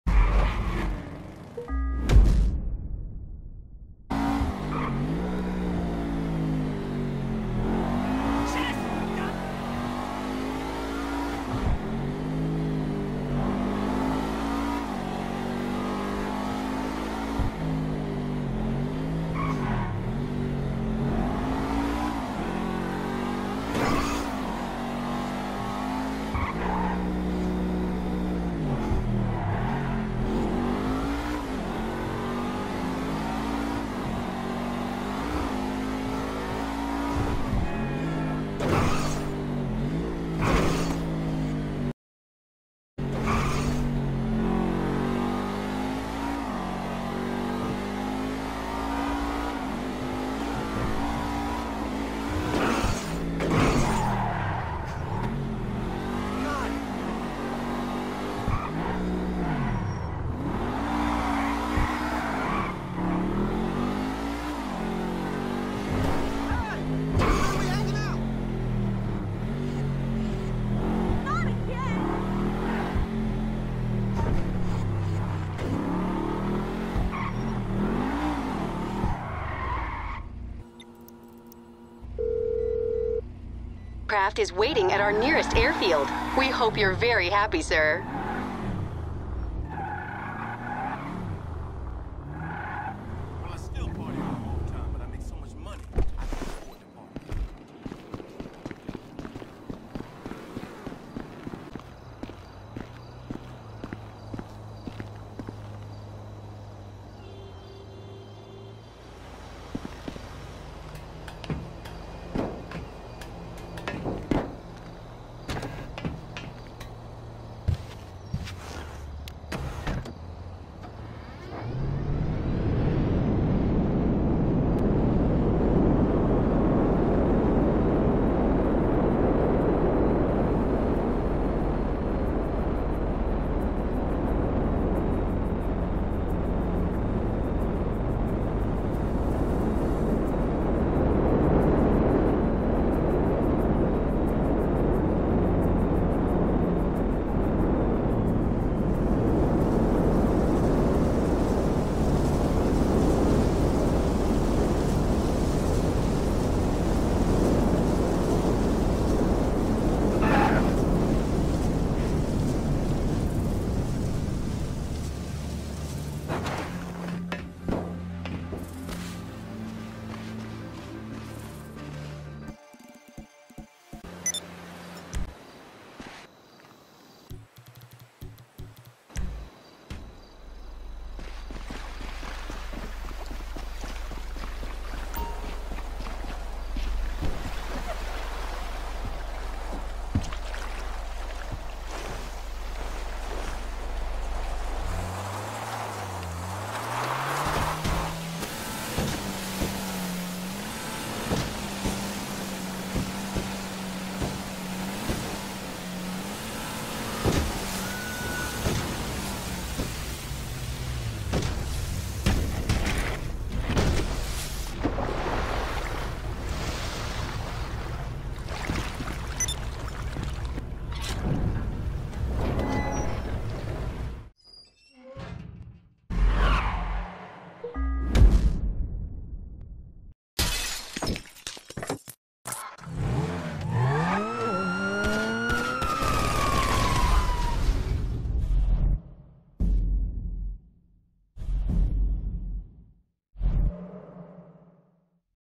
You Finished 1st -- s1e0 -- Commute to My New Home Base [1st person view]
Here is what I do by default to set up the next mission -- a preview of the hardware to come.